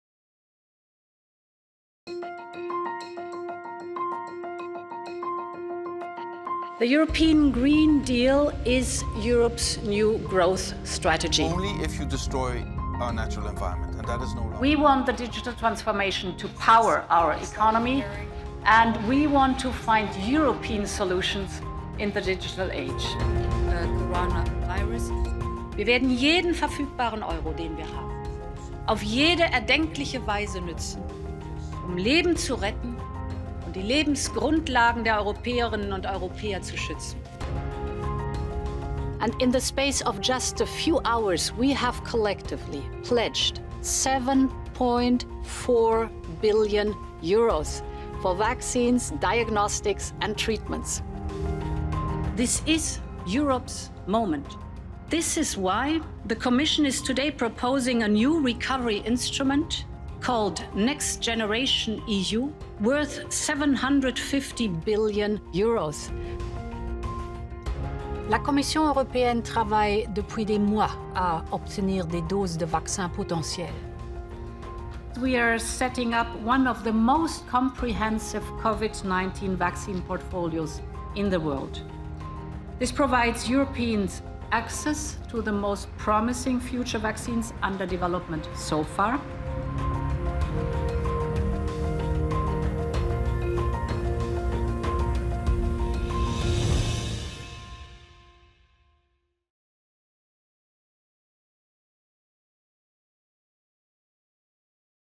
1st year anniversary: von der Leyen's Commission
Today, Ursula von der Leyen celebrates her 1st anniversary as President of the European Commission. Do you know how the Commission works? Due to Brexit misinformation, many people across the UK have no idea. Read my report to find out more